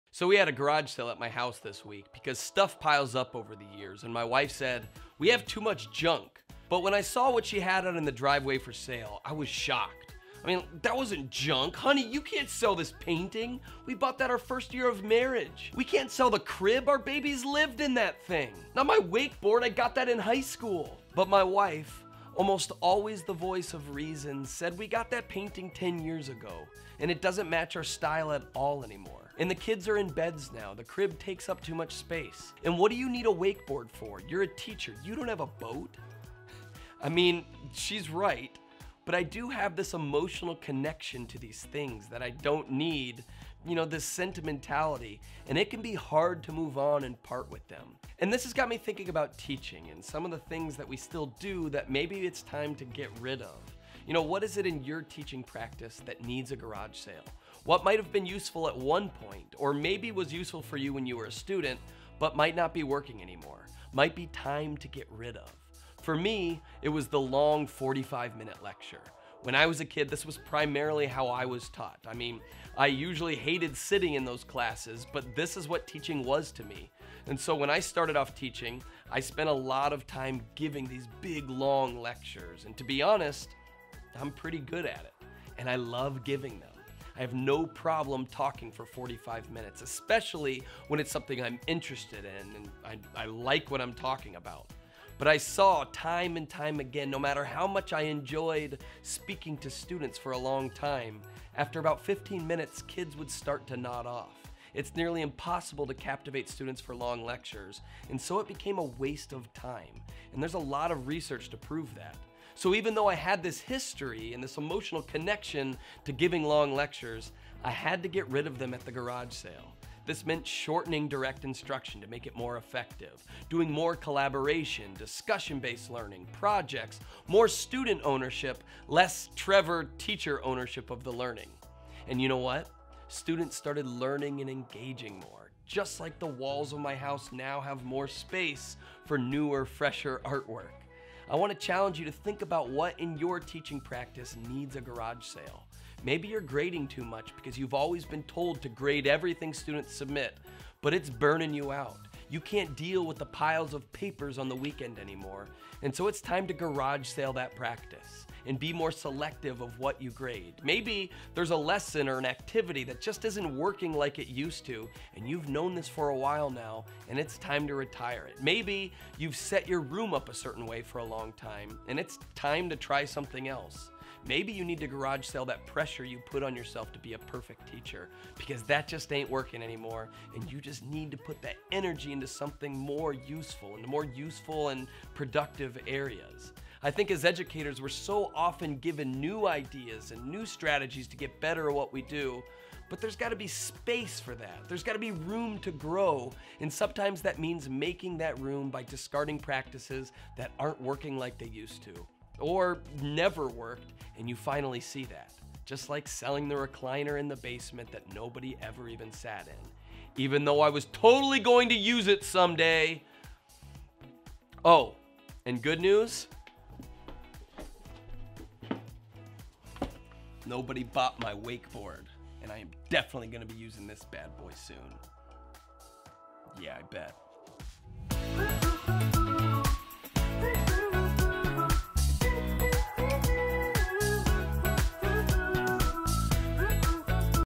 Getting Rid of Teaching Practices That Don't Work
What is it in your teaching practice that needs a garage sale? What might have been useful at one point, or maybe was useful for you when you were a student, but might not be working anymore- might be time to get rid of?